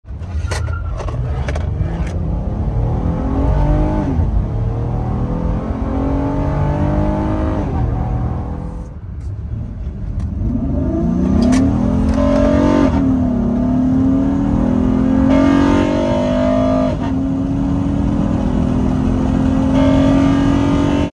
2018 Honda Pilot Before & After HPS Intake Install. LOUD VTEC!
Here is a before and after run inside my 2018 Honda Pilot. I installed an HPS intake to replace the stock airbox. What a difference it makes! Love the sound! Added some fun to my daily family hauling vehicle.